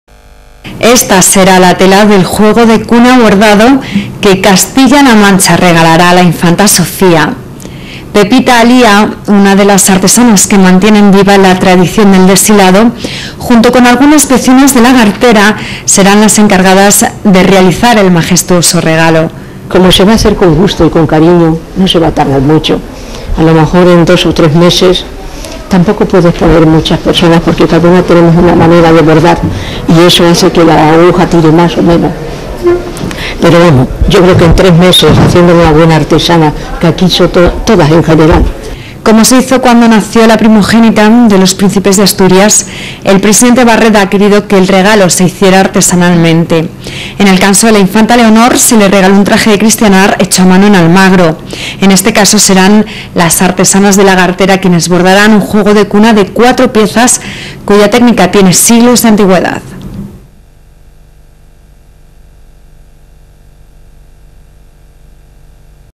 Regalo Infanta Doña Sofía
El presidente de Castilla-La Mancha, José María Barreda, ha conocido el regalo que las artesanas de Lagartera, en Toledo, van a realizar a la segunda hija de los Príncipes de Asturias, la Infanta Doña Sofía, en nombre del gobierno regional.